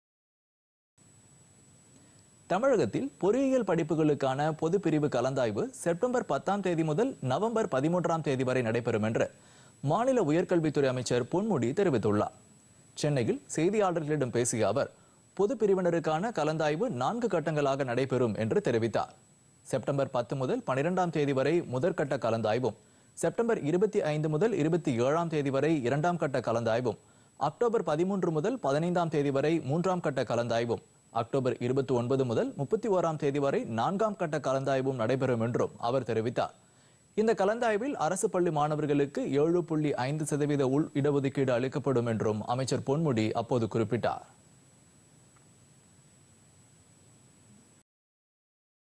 செப்.10-ல் பொதுப்பிரிவு கலந்தாய்வு
தமிழகத்தில் பொறியியல் படிப்புகளுக்கான பொதுப்பிரிவு கலந்தாய்வு செப்டம்பர் 10 ஆம் தேதி தொடங்கும்- மாநில உயர்கல்வித்துறை அமைச்சர் பொன்முடி அறிவிப்பு